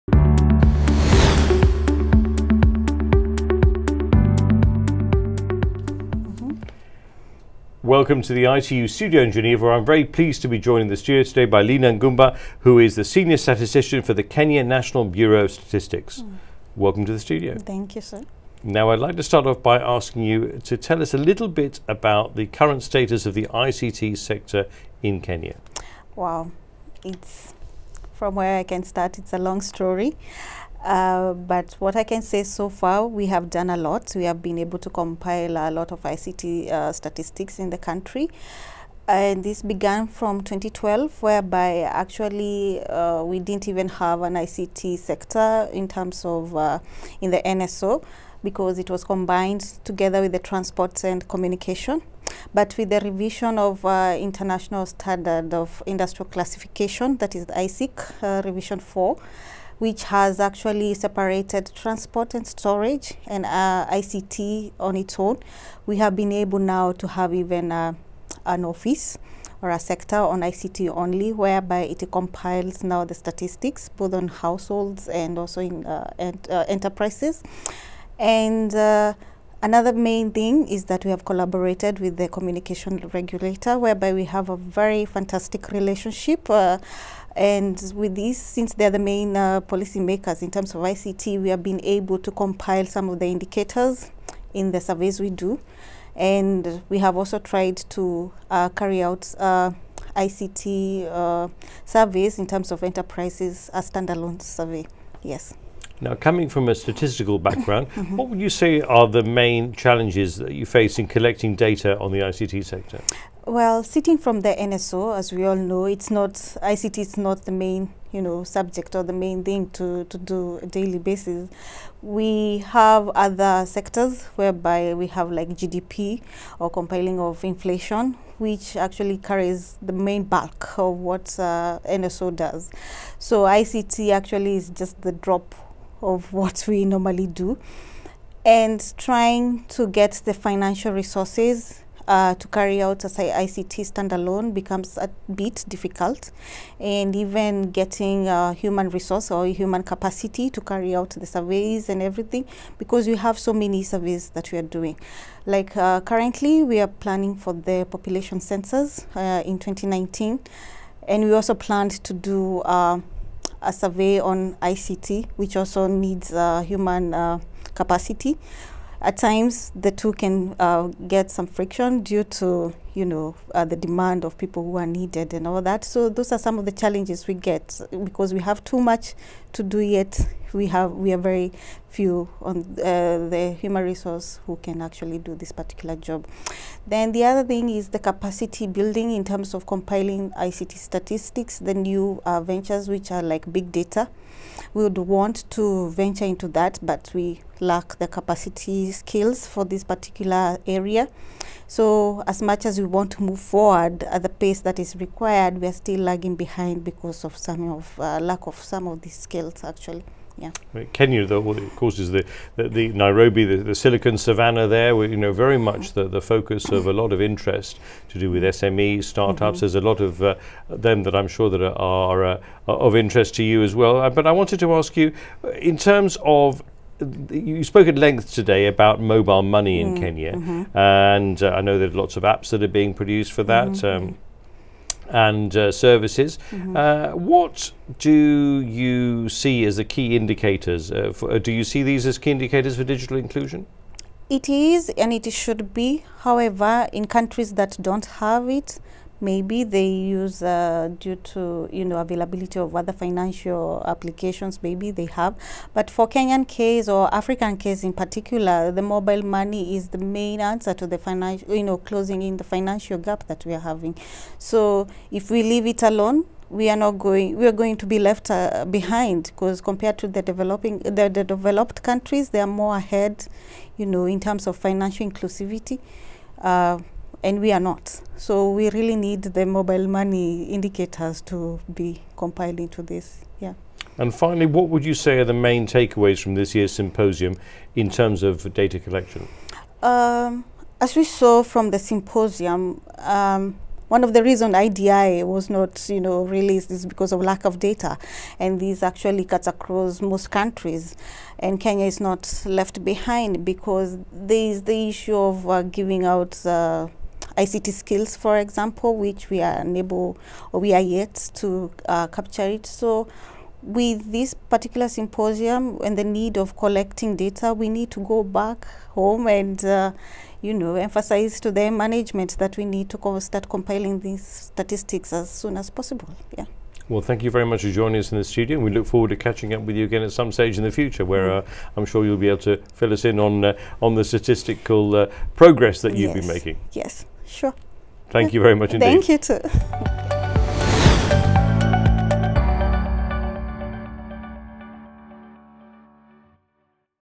ITU INTERVIEWS @ WTIS-18: Linah Ngumba, Head, National Statistics Office, Vice-Chair (EGH), Kenya
Interview with Linah Ngumba, Head, ICT Statistics Section, National Statistics Office, Vice-Chair of the Expert Group on Households (EGH), Kenya

WTIS-18 features high-level debates on the impact of telecommunications/ICTs and emerging technologies on social and economic development. Other sessions of WTIS-18 will focus on measurement in an environment of artificial intelligence, big data, robotics and the Internet of Things. The work of the Expert Group on Telecommunication/ICT Indicators (EGTI) and the Expert Group on Household Indicators (EGH) will also be presented for adoption by WTIS-18. The Symposium also looks at how to measure ICT skills and how progress can be measured on implementing the Sustainable Development Goals (SDGs).